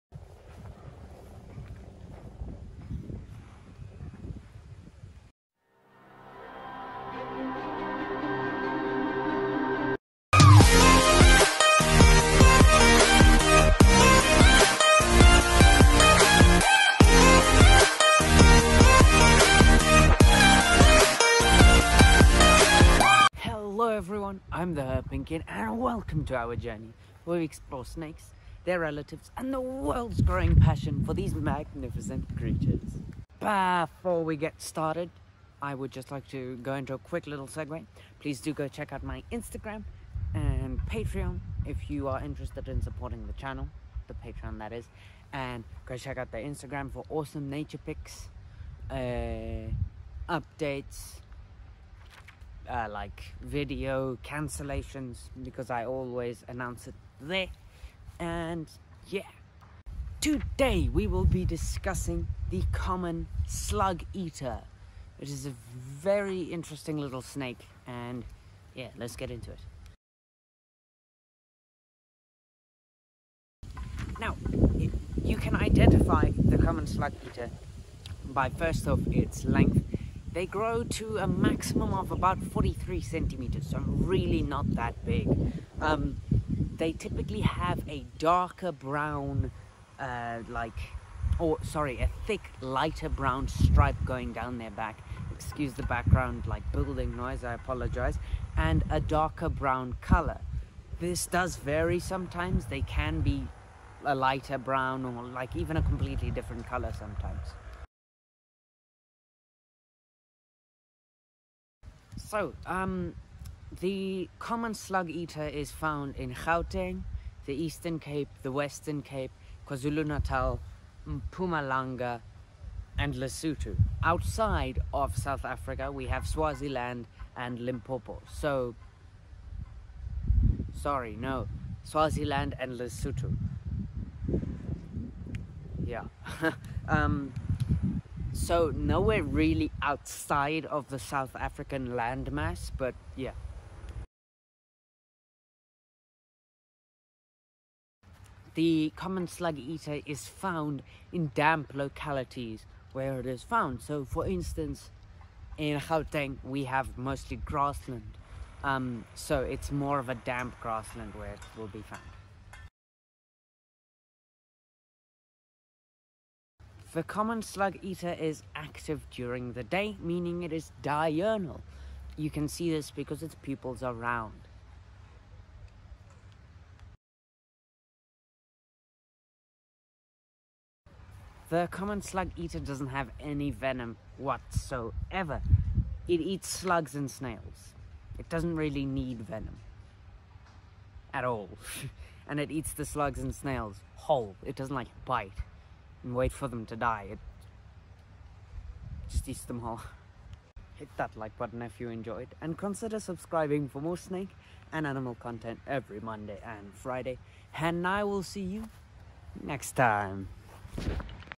Snake Profile: Common Slug-Eater
Welcome Snake Lovers!
Today we'll be profiling and discussing the Common Slug-Eater!

The Association of Environmental Issues Instagram!

🕊️Consider following my twitter!